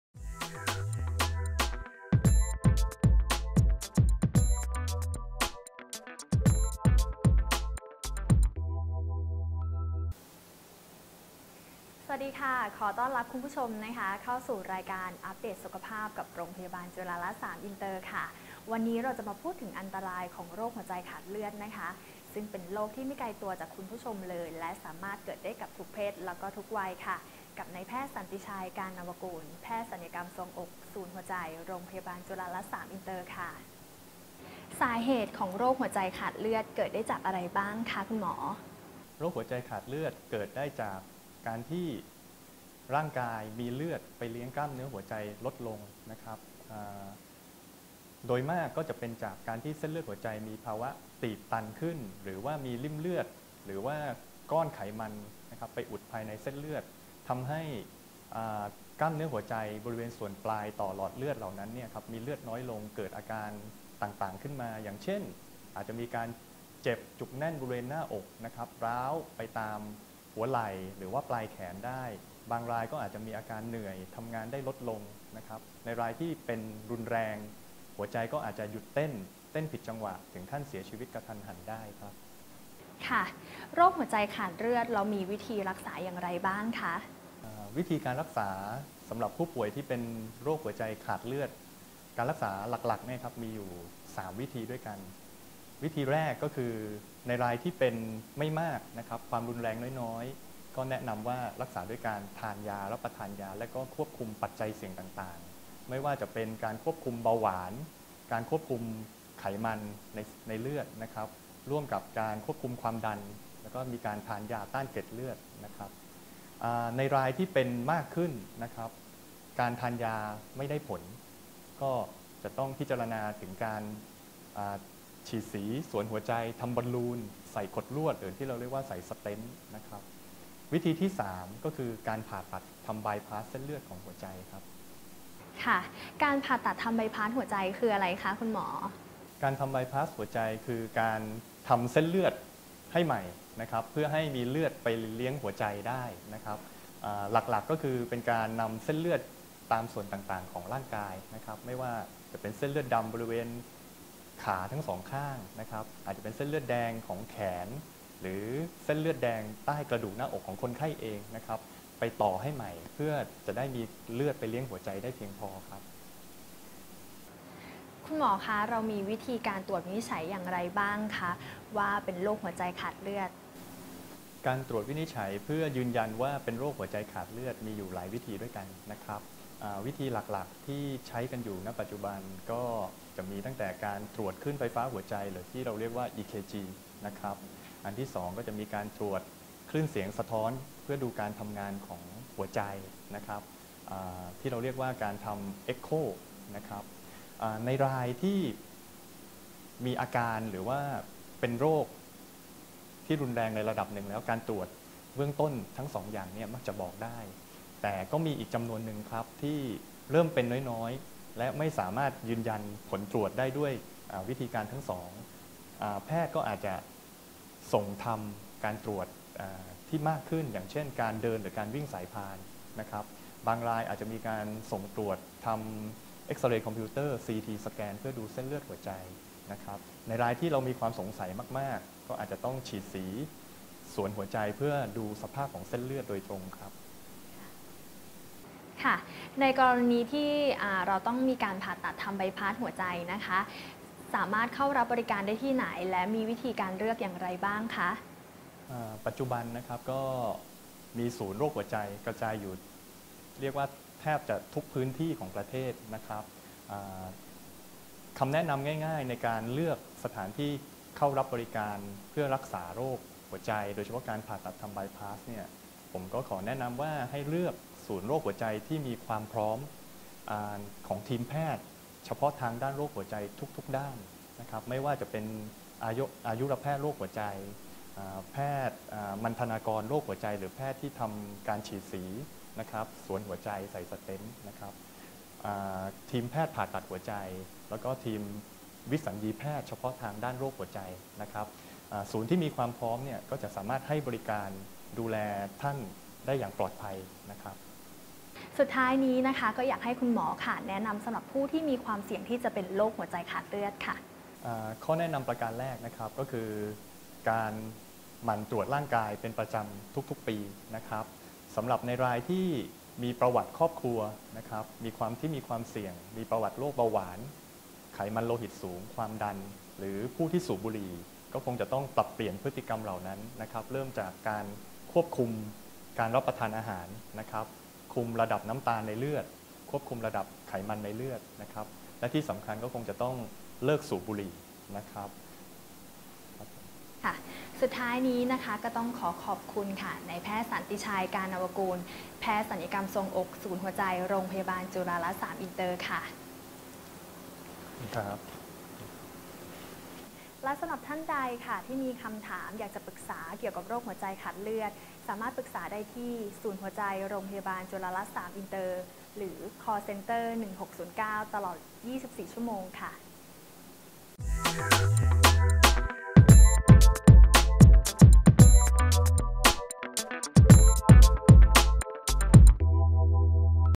รายการอัพเดทสุขภาพ "ภัยร้ายโรคหัวใจ"
โดย นพ.สันติชัย  กาลเนาวกุล
หัวหน้าหน่วยศัลกรรมทรวงอก ศูนย์โรคหัวใจ
รพ.จุฬารัตน์ 3 อินเตอร์